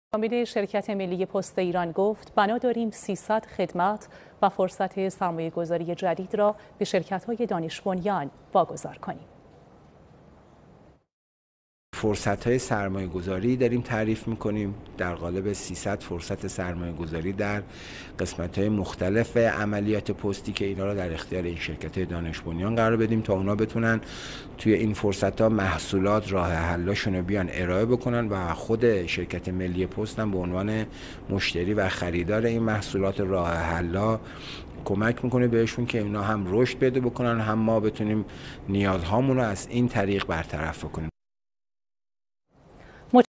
مدیرعامل شرکت ملی پست ایران - شبکه_خبر - اخبار علم و فناوری
شبکه_خبر اخبار علم و فناوری
 17:43 1401/05/13
 مدیرعامل شرکت ملی پست ایران : بنا داریم 300 خدمت و فرصت سرمایه گذاری جدید را به شرکت های دانش بنیان واگذار کنیم